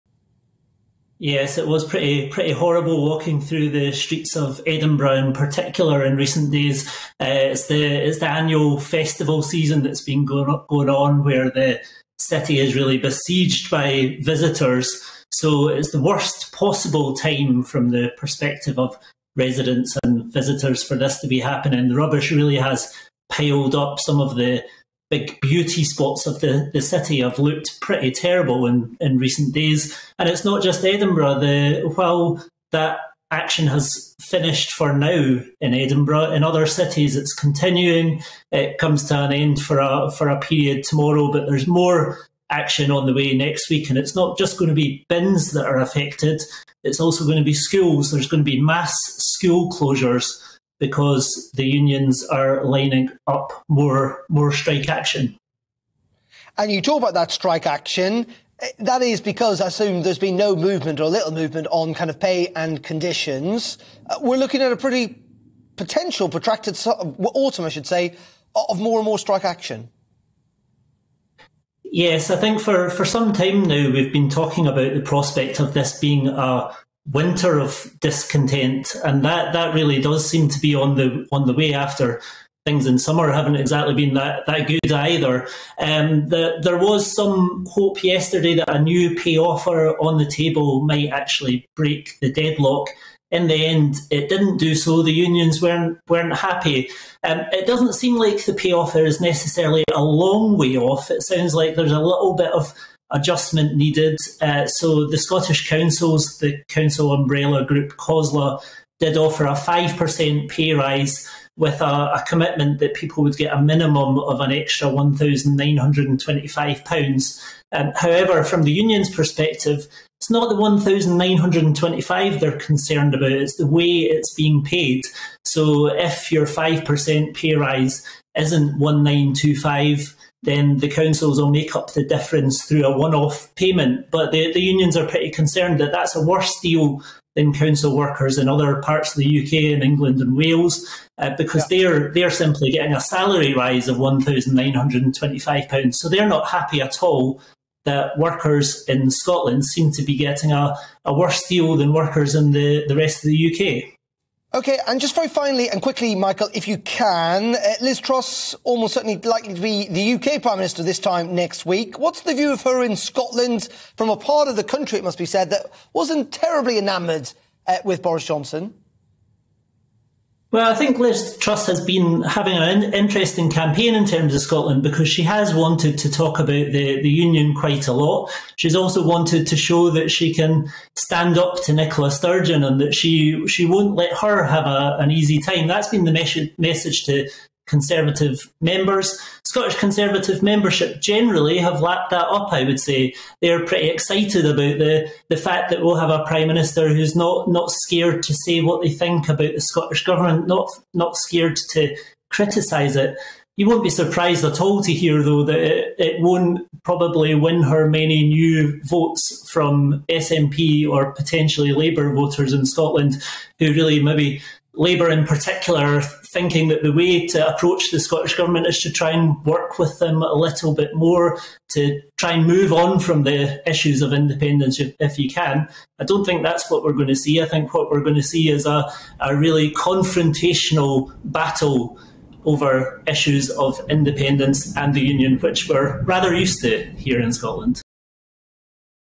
Refuse worker strikes: 'It doesn't sound like the pay offer is a long way off' | Michael Blackley
'It doesn't sound like the pay offer is a long way off... the Scottish Councils group, COSLA, did offer a 5% pay rise.'

Scottish Daily Mail Political Editor Michael Blackley talks about refuse worker pay rise discussions taking place, as waste services resume in Edinburgh after strikes.